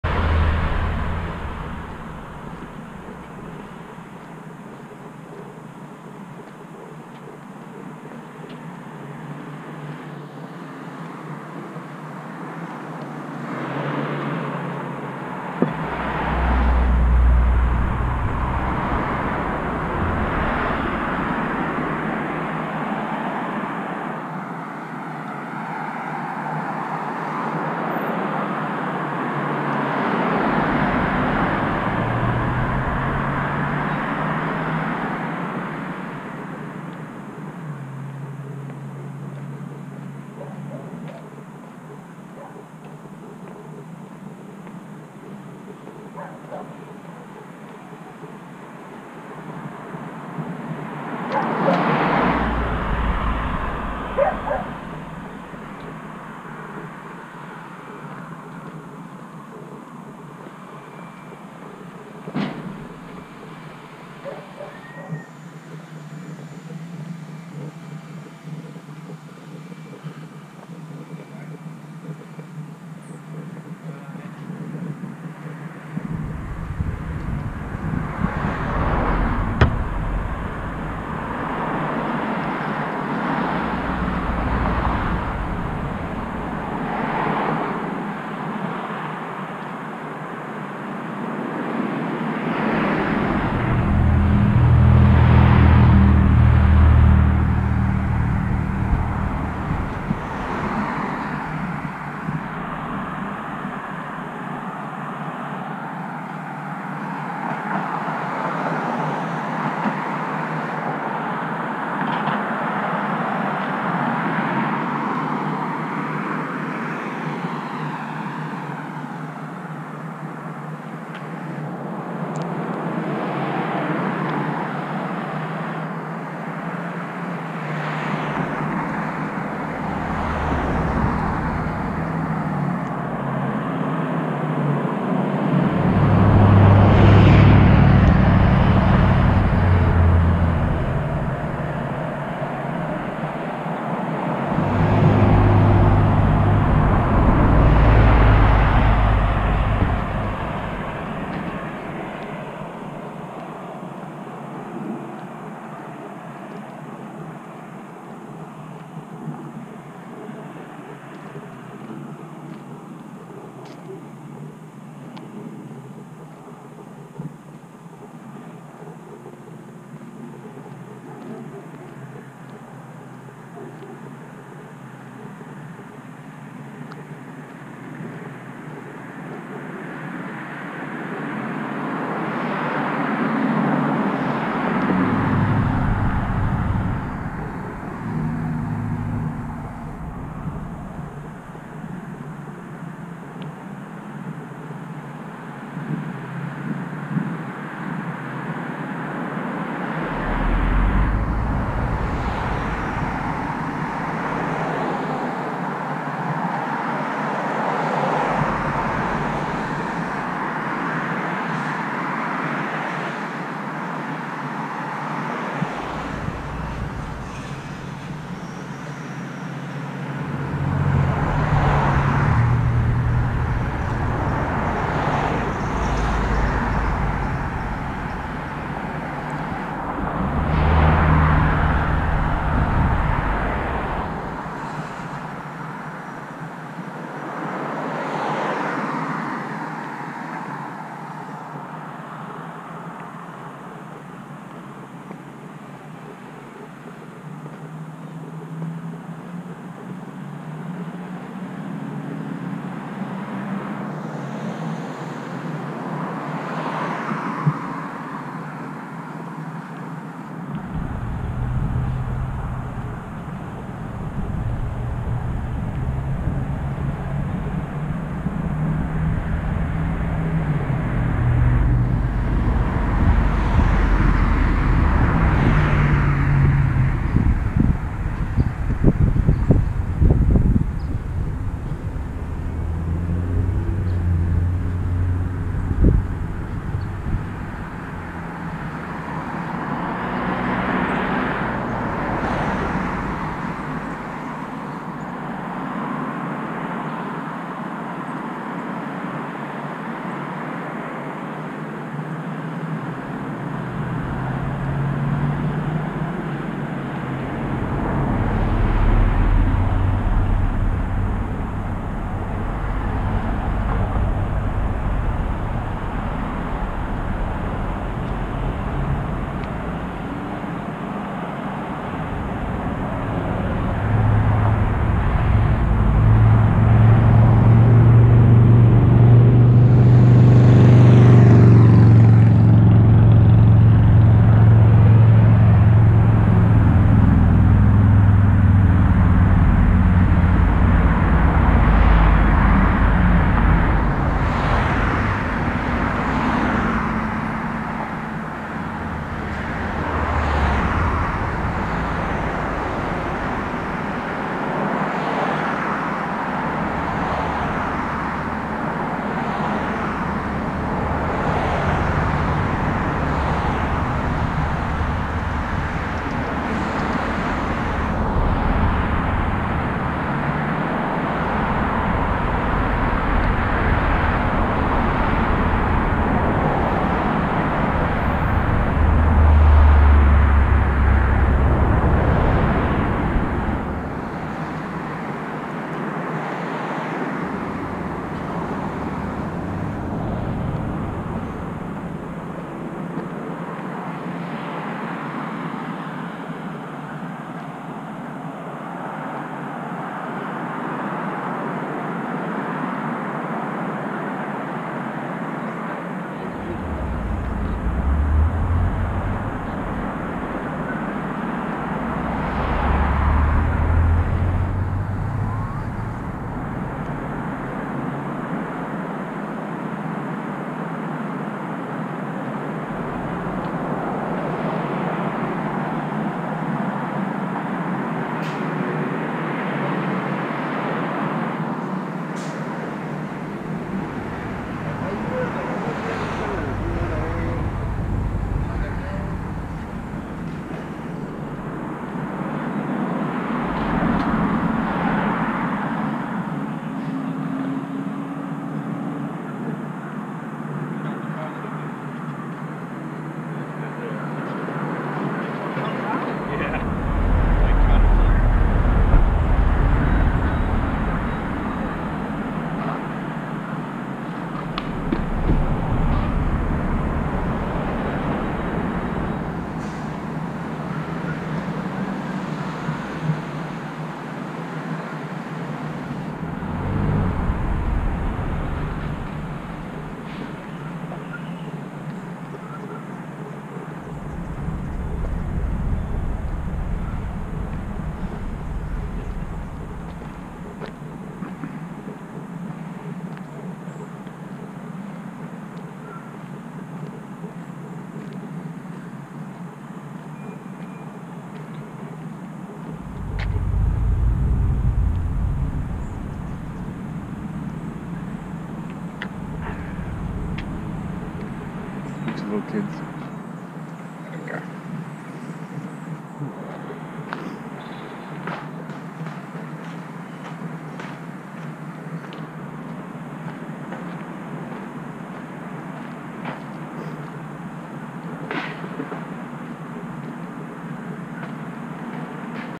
Peck Park, San Pedro || Part 3
Walk in Peck Park.  Going past the transition skate park to the trail along Peck Canyon on the south side of the park.  I end at the entrance on the north side of the park.